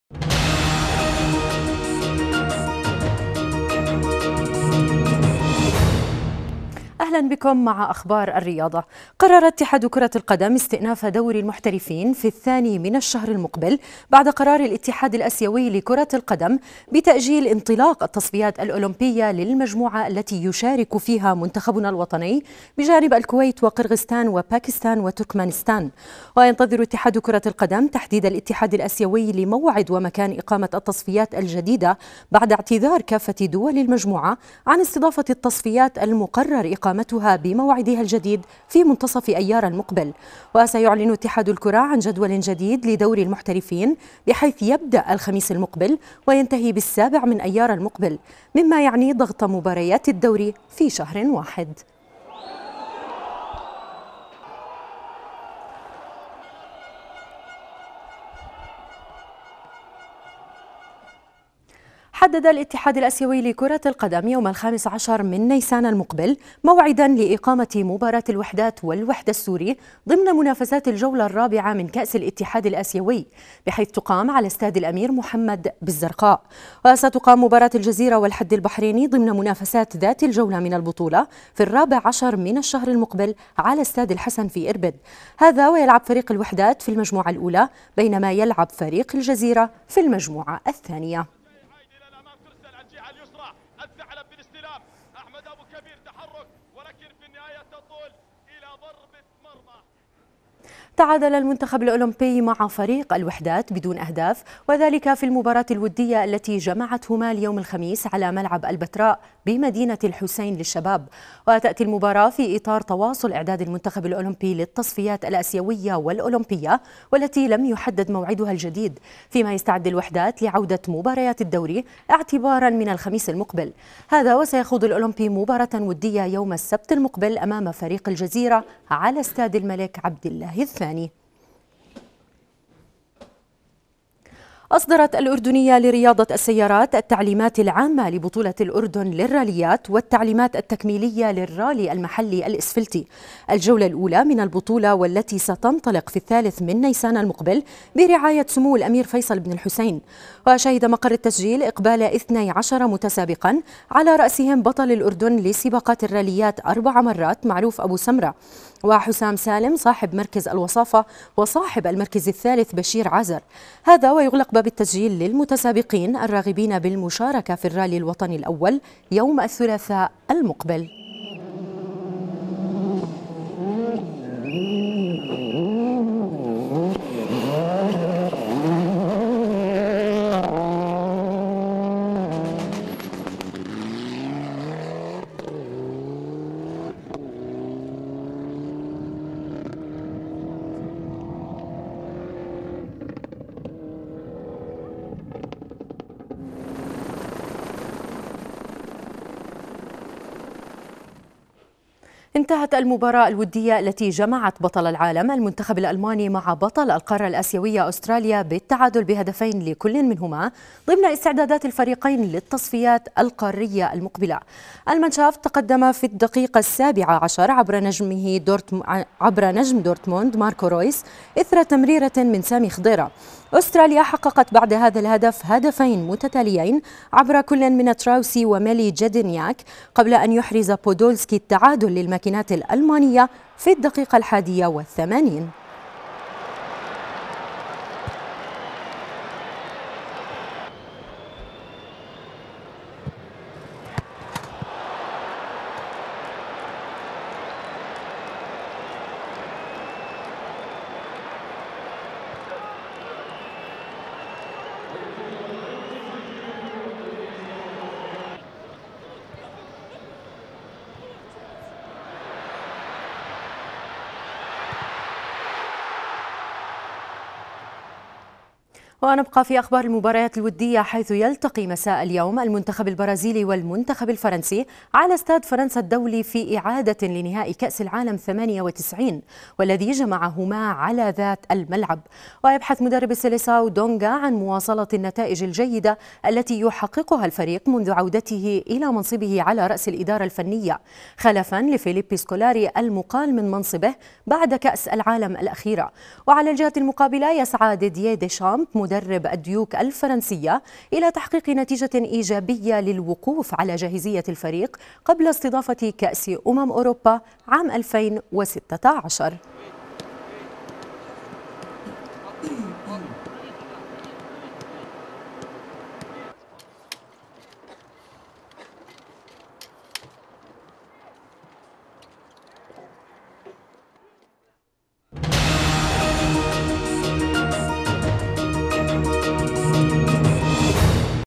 النشرة الرياضية 26-3-2015
النشرة الرياضية التي قدمتها الزميلة سميحة مجدلاوية ليوم 26/3/2015.